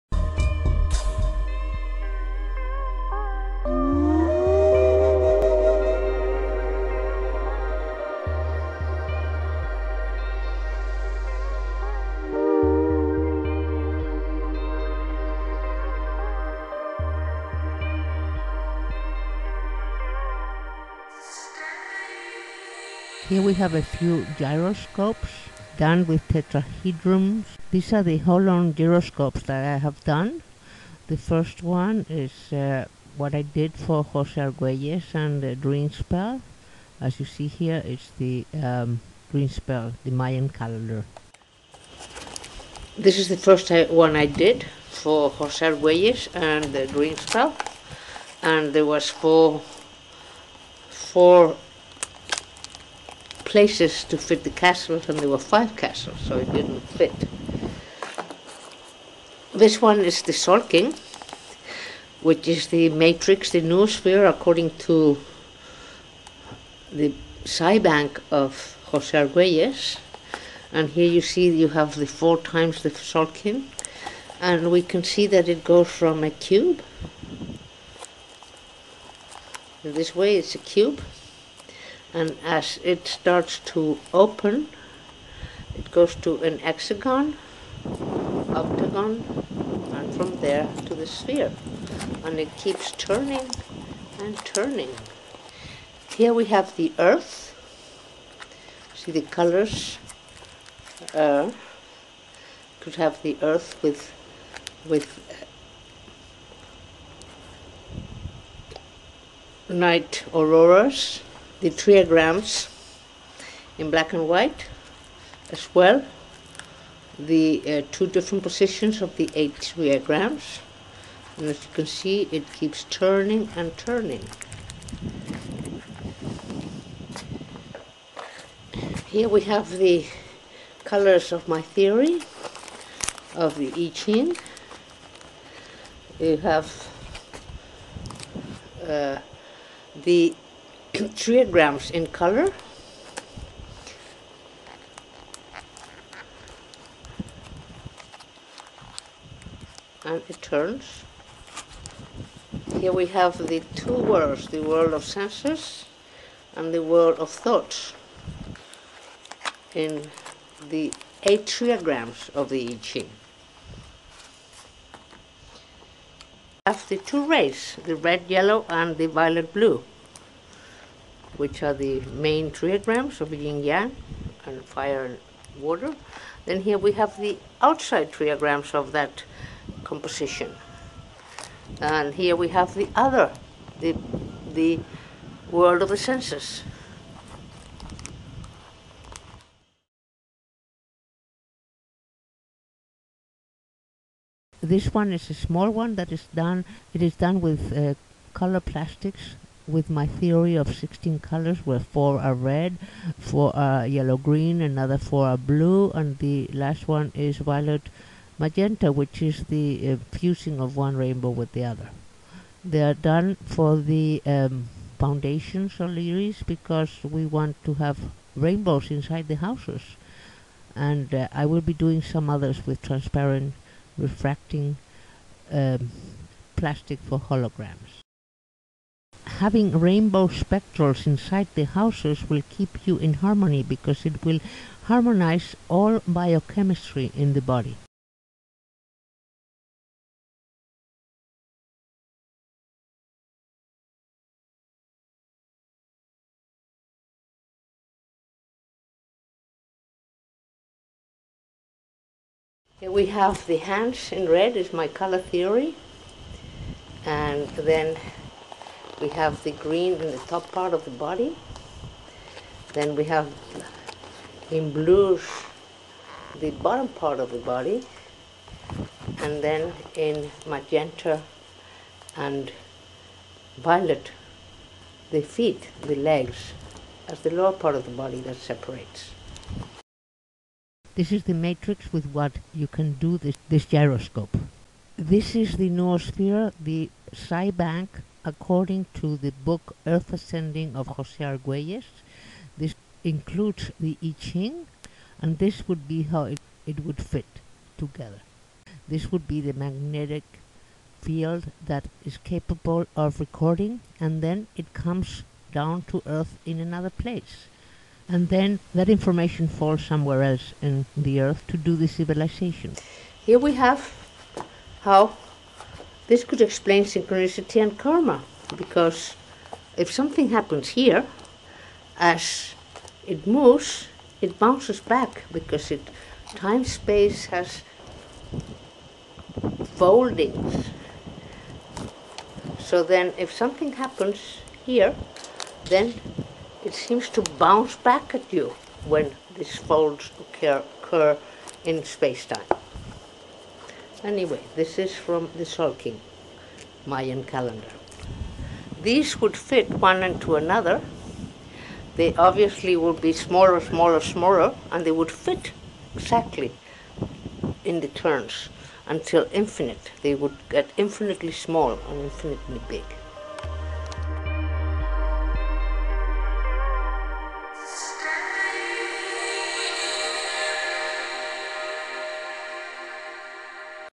Holon Tetrahedron GYROSCOPE
This is the holon tertrahedron gyroscope that is made with cardboard. It is an Origami is a Japanese, a paper sculpture. It reminds me of Nassim Haramein description of our universe and the finger of God of Marko Rodin.
I will continue doing a large video about my theory TAO universe.